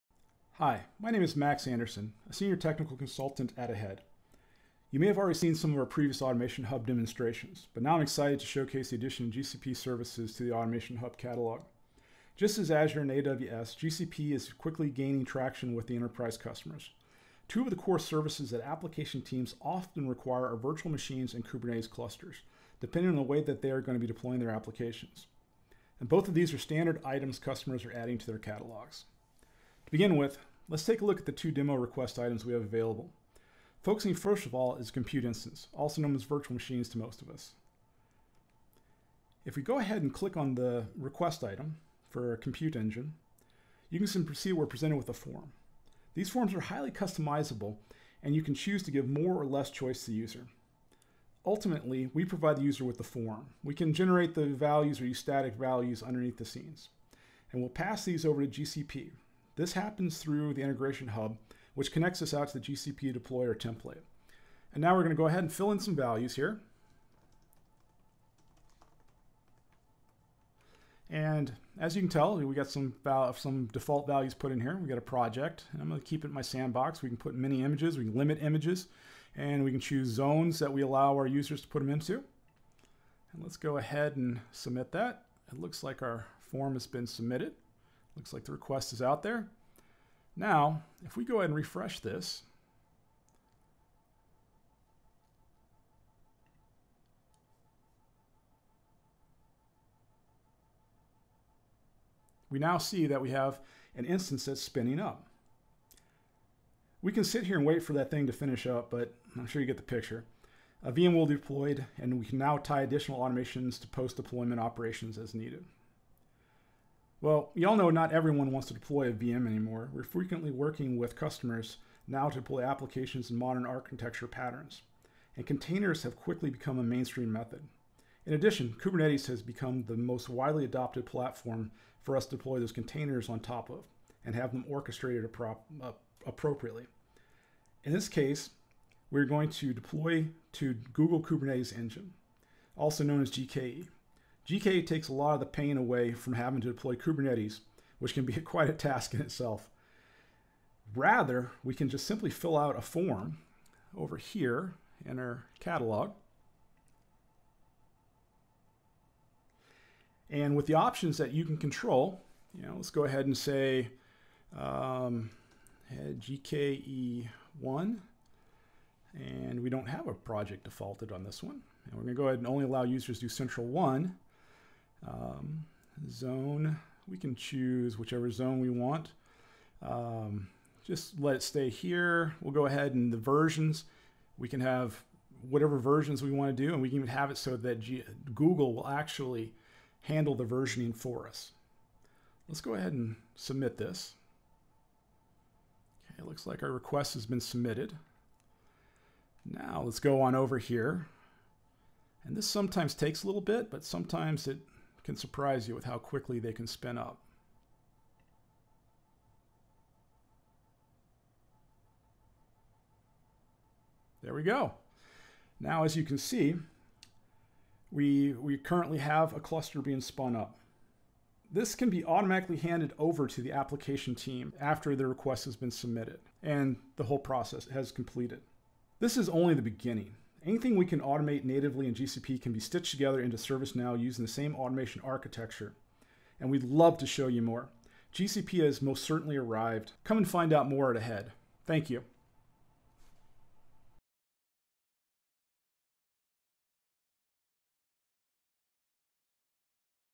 Squeeze More Efficiency from GCP with AHEAD Automation Hub
AHEAD’s Automation Hub helps IT teams deliver cloud services faster and more efficiently, including within Google Cloud Platform. This demo explores virtual machines and Kubernetes clusters within Automation Hub for GCP.